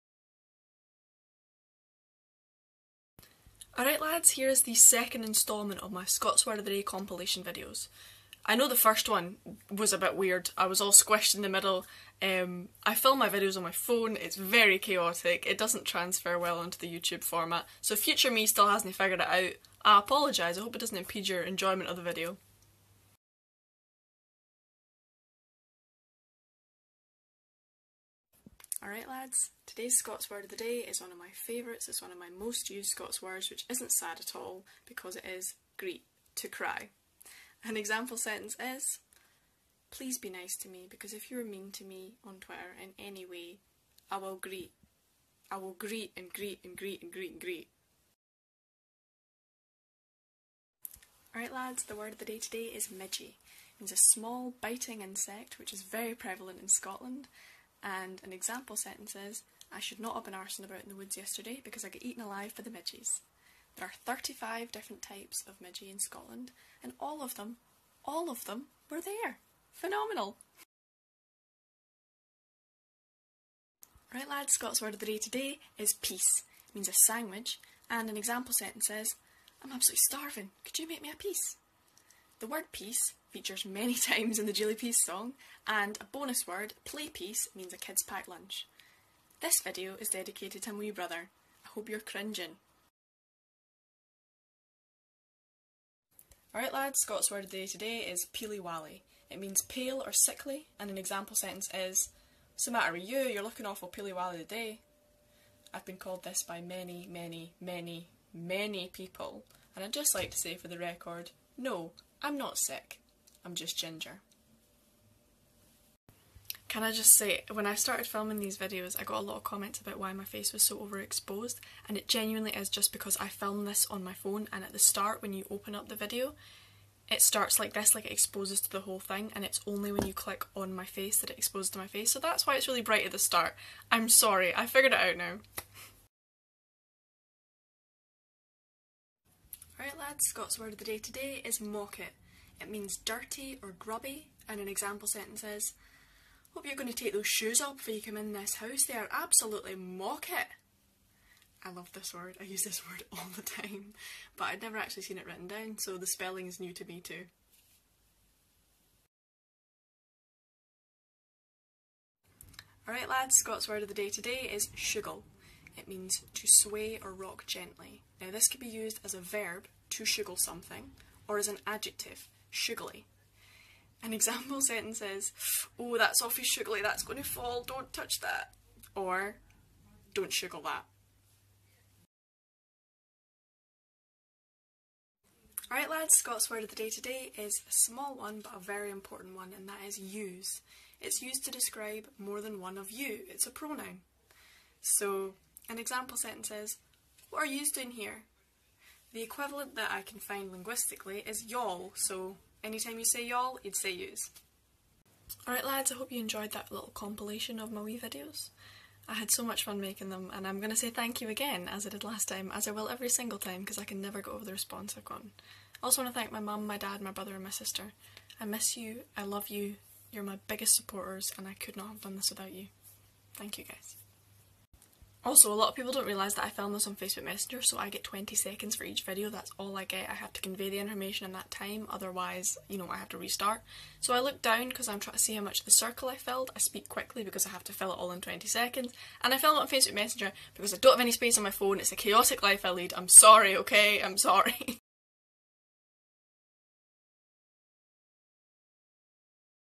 Scots word of the day Volume 2: 9th- 15th July 2020
Alright lads, here's the second instalment of my compilation videos. Bear with me while I work out the kinks, I'm learning how to edit and make videos as I go so things might be rough for a wee while.

Greet

Midge

Piece

Peely-wally

Mawkit

Shooogle/shoogly

Yous

As always, thank you so much for taking the time out of your day to watch, I'm so grateful.

Lennie x